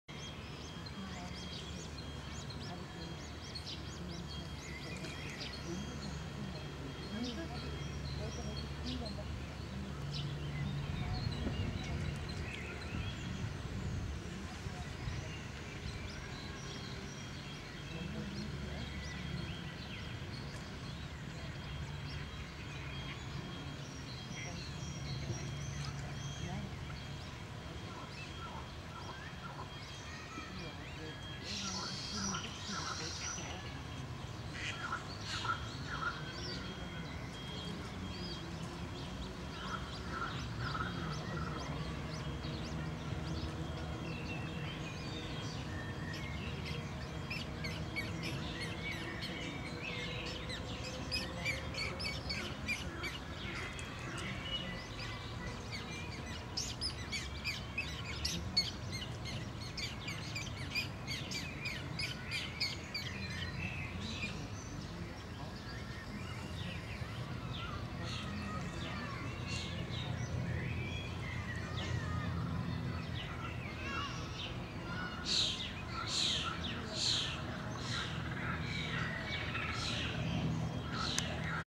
Grimnitzsee Haubentaucher
Wee black baby ducks on Grimnitzsee in Berlin.
Mum is diving to get food for them.